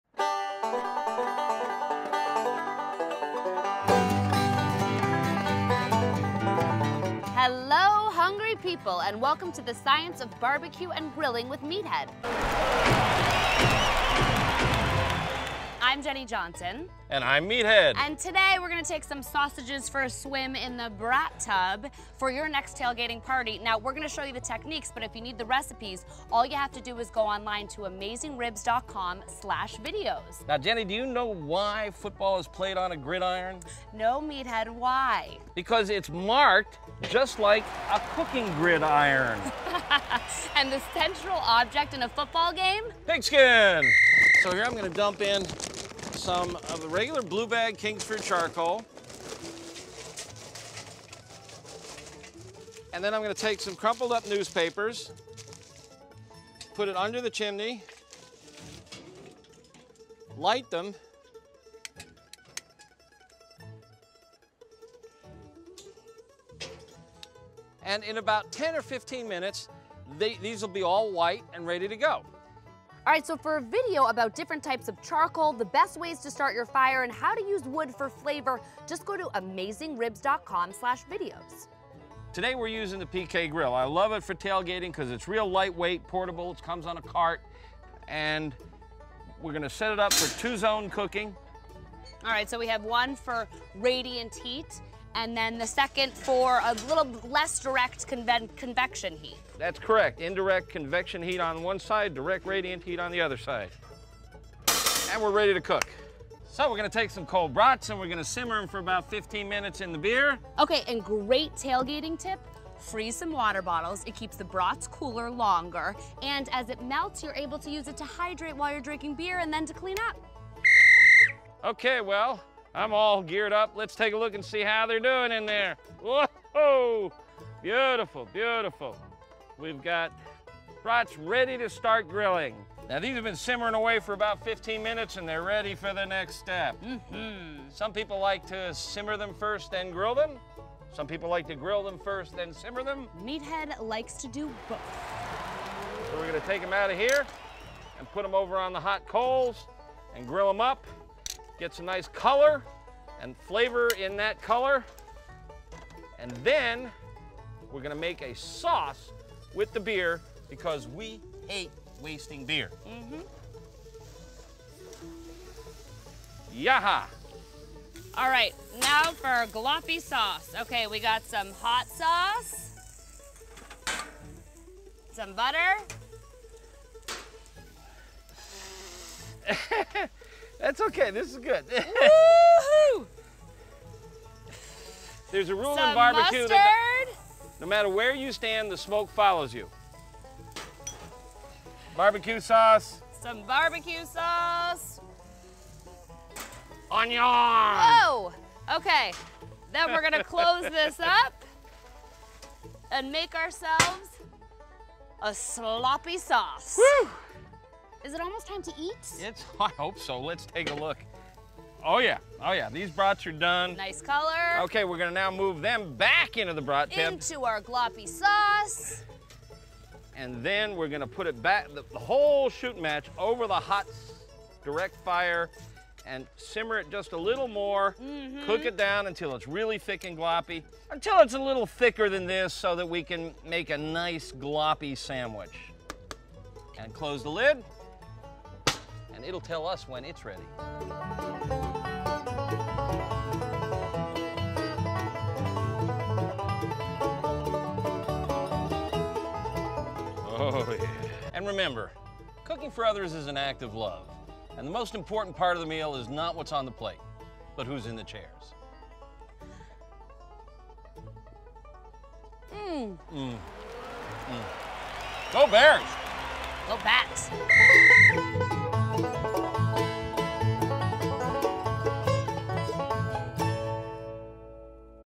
Improved Brat Tub
Here's how I amp up the boring old Brat Tub by making a sauce with the beer and then making beer cheese too!